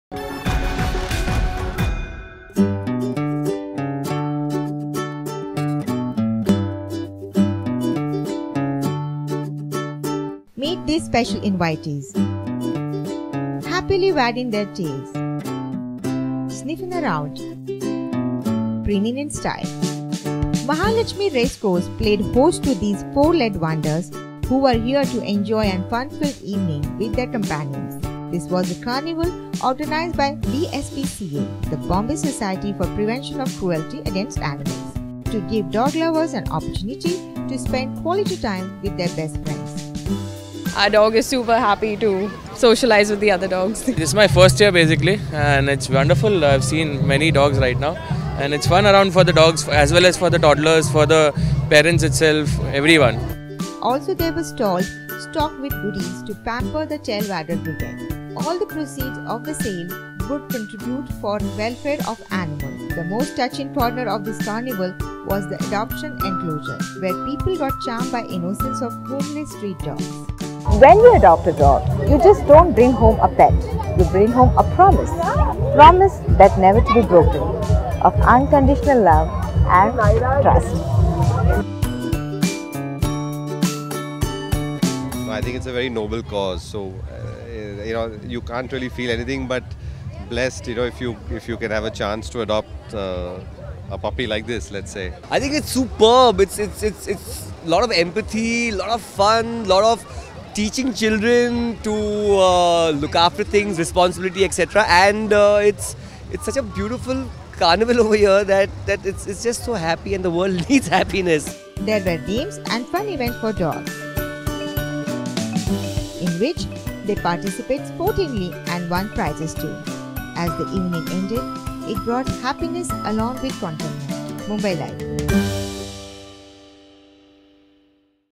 A Dog's day out
Racecourse-Mahalaxmi race course played host to these four legged wonders who were here to enjoy a funfilled evening with their companions. This was  a carnival organised by BSPCA (The Bombay society for prevention of cruelty against animals) to give dog lovers an opportunity to spend quality time with their best friend.
Also there were stalls stalked with goodies to pamper the tail wagging brigade . All the proceed of the sale would contribute for welfare of animals. The most touching corner of this carnival was the adoption enclosure, where people got charmed by the innocence of homeless street dogs.